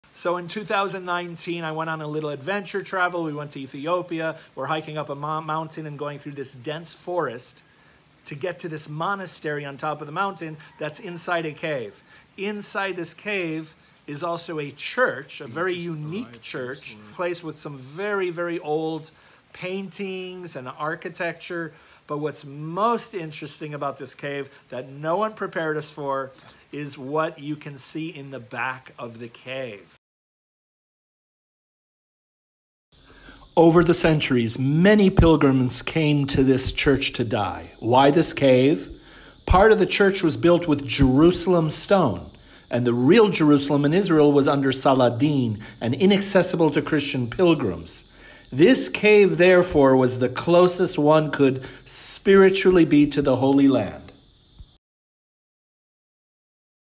A Church Full of Mummies!!!!
A simple hike to a monastery in Ethiopia turned into a surprise that we didn’t see coming.